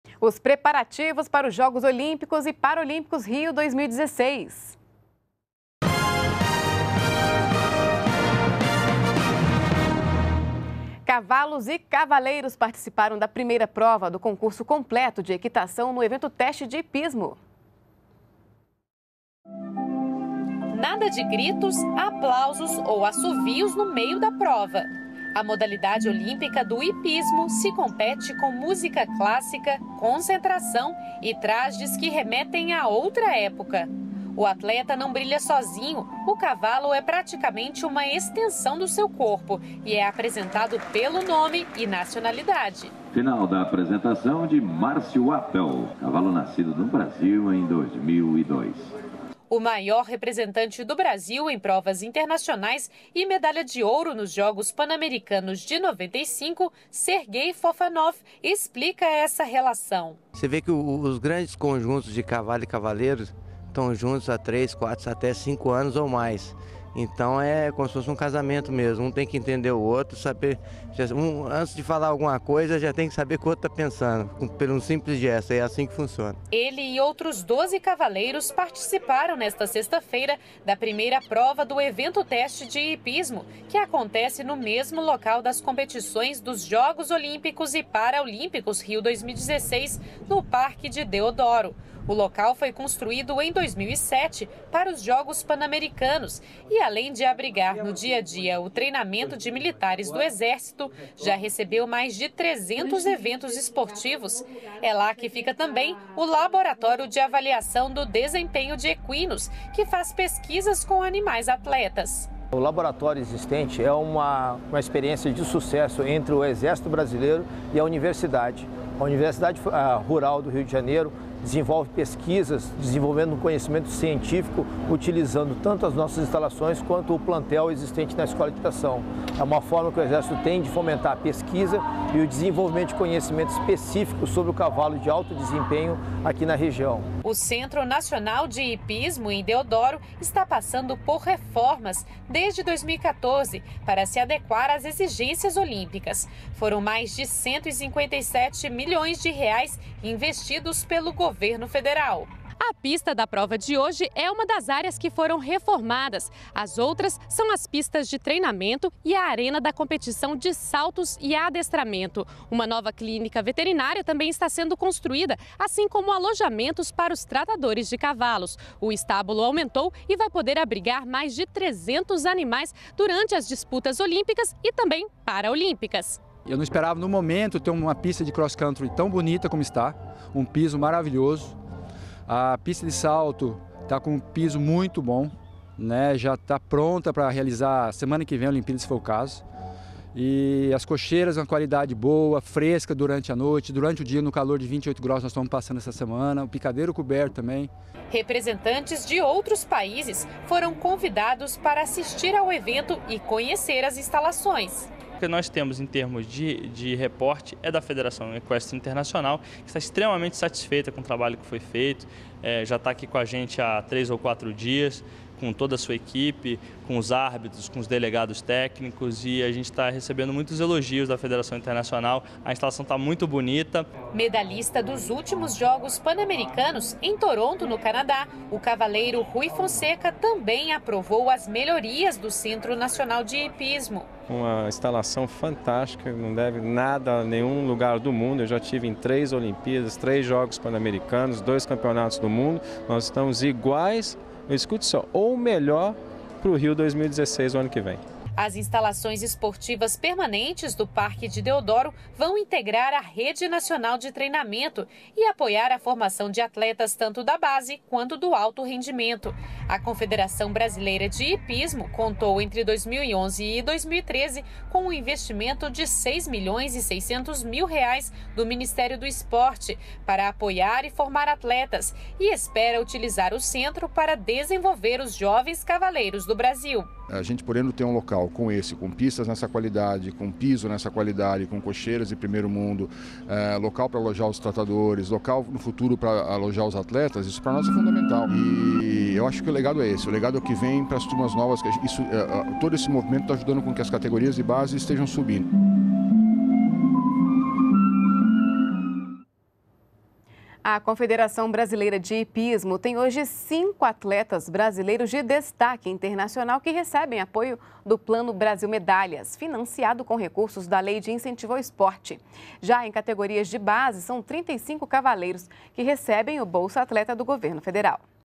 Parque de Deodoro recebe primeiro evento teste de hipismo dos jogos Rio 2016
NBR NOTÍCIAS - 07.08.15: Cavalos e cavaleiros participaram da primeira prova do concurso completo de equitação, no evento teste de hipismo, durante os preparativos para os Jogos Olímpicos e Paraolímpicos do Rio 2016. O maior representante do Brasil em provas internacionais de hipismo é Sergei Fofanoff, que explica a relação entre cavalos e cavaleiros para o melhor aproveitamento das provas. Participaram da primeira prova de teste, no Centro Nacional de Hipismo de Deodoro, no Rio de Janeiro, 13 competidores.